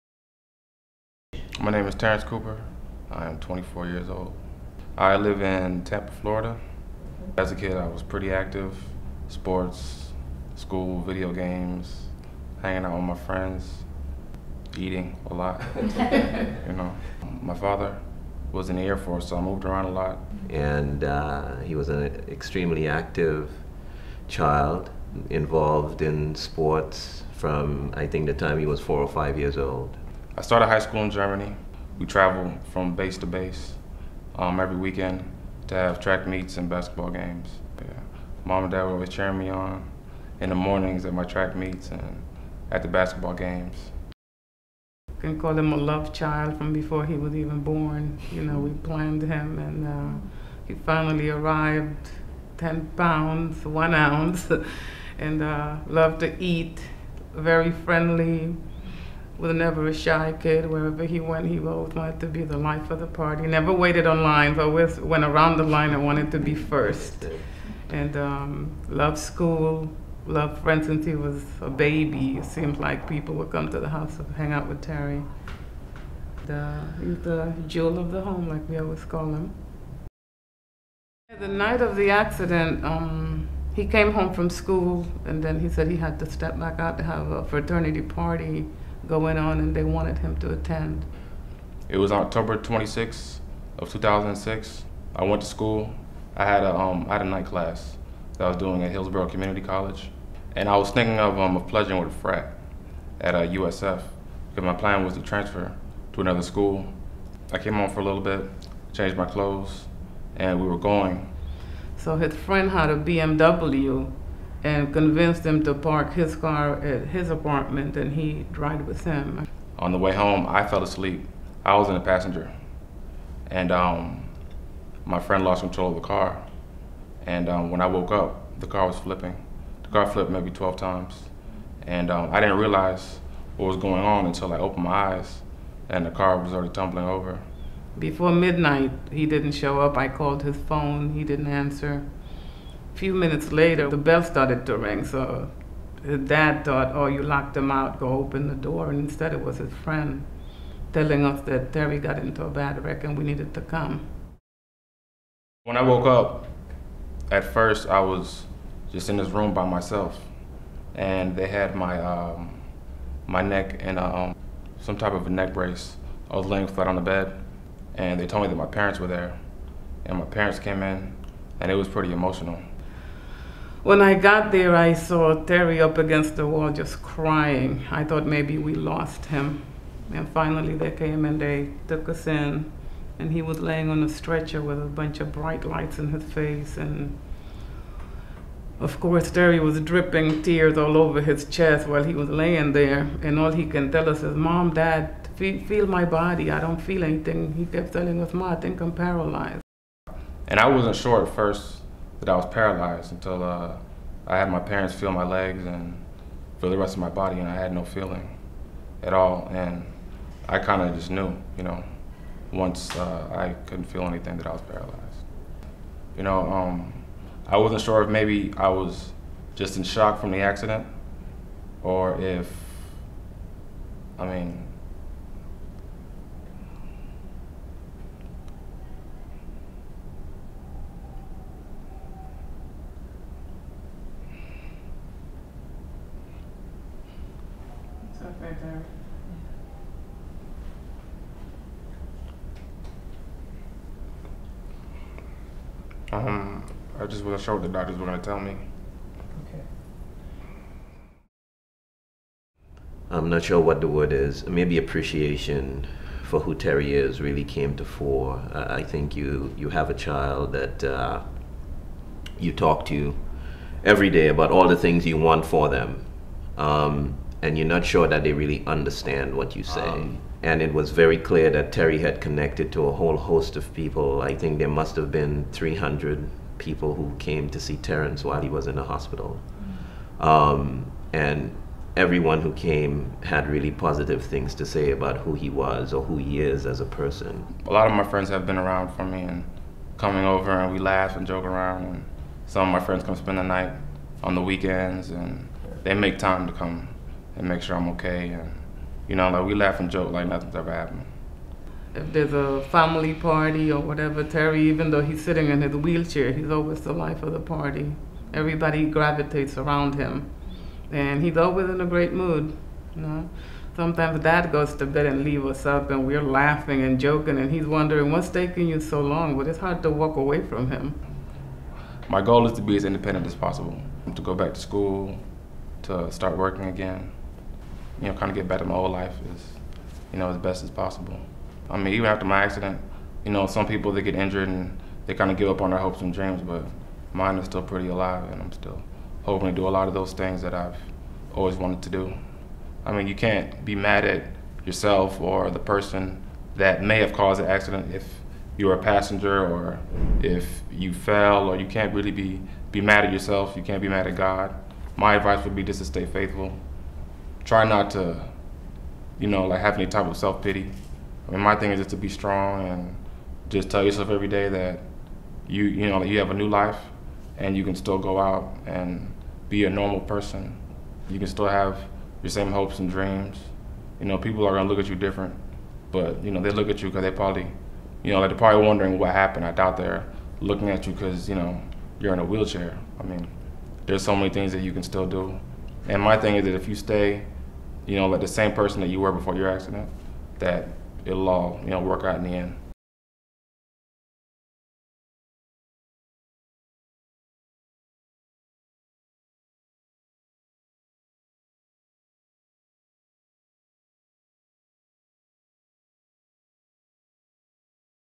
Terrence Cooper - Race to Independence (Extended Version)
The story of Terrence Cooper, his accident, rehabilitation, and hopes for the future.